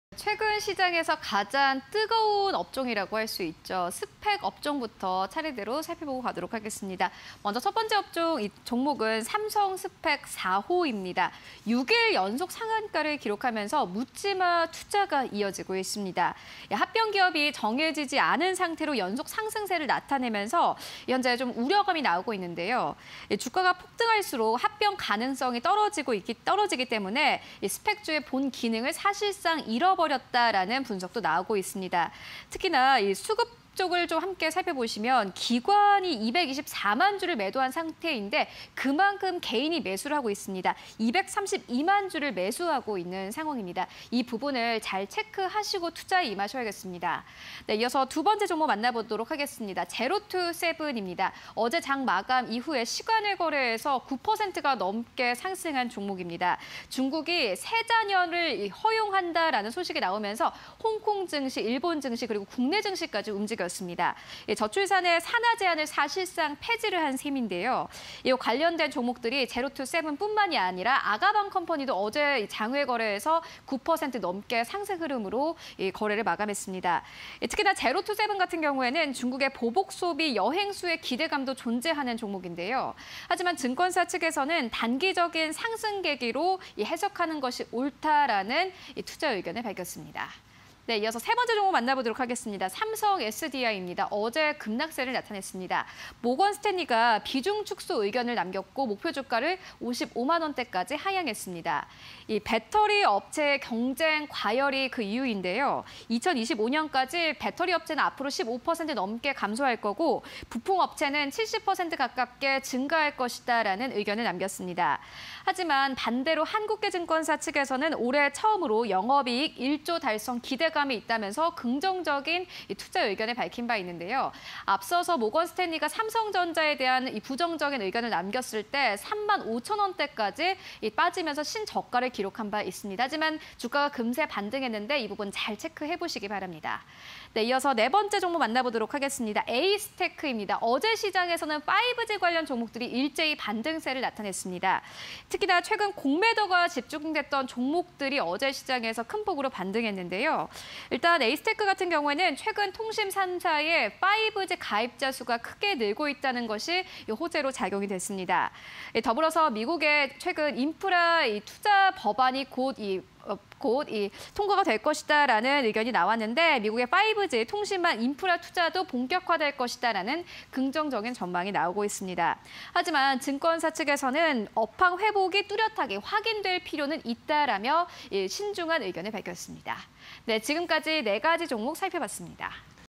삼성스팩4호, 제로투세븐, 삼성SDI, 에이스테크_공시&뉴스 (20210601)
- 6일 연속 상한가… ‘묻지마 투자’ 이어져
- 합병 기업 정해지지 않은 상태로 연속 상승세
- 주가 폭등할수록 합병 가능성 떨어져
- 개인 232만주 매수 vs 기관224만주 매도

- 중국, 3자녀까지 허용… 저출산에 산아제한 사실상 폐지
- 中 보복소비 여행 수혜 기대감 존재
- 단기적 상승 계기로 해석하는 것이 옳다는 견해도

- 모건스탠리 ‘비중 축소’ 의견에 주가 하락
- 한국계 증권사 “올해 처음으로 영업이익 1조 달성 기대감”

- 최근 공매도 집중된 종목 저평가 매력 부각
- 통신3사 5G 가입자수 증가
- 미국 5G 통신망 인프라 투자 본격화
- 뚜렷한 업황 회복 확인할 필요 있어